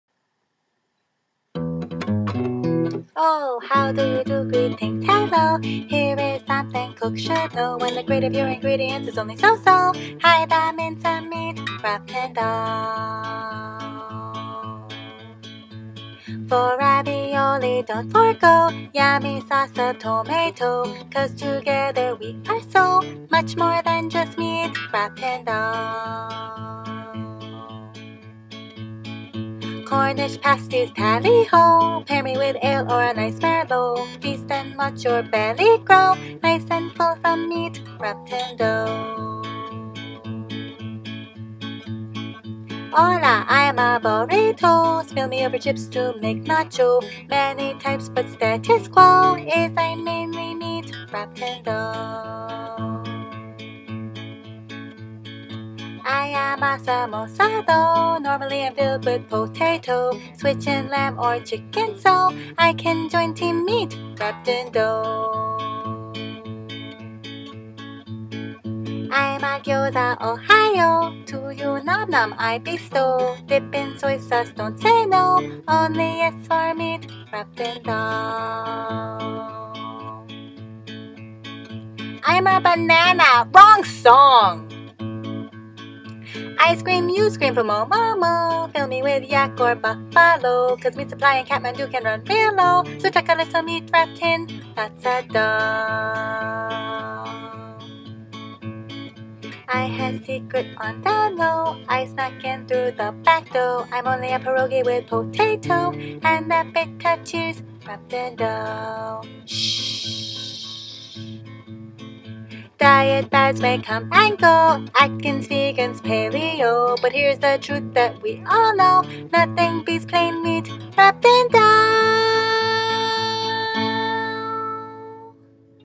Meat Wrapped in Dough
Culinary innovation began only when hominids discovered the art of wrapping meat (a #1 or #2 food) in dough (also a #1 or #2 food). I don't care which side your country fought on during WWII, or if your culture didn't even know there was mad fighting transpiring: I bet you all the vegetables in the world that soldiers, civilians, profiteers and even some women were eating meat wrapped in dough. Respect.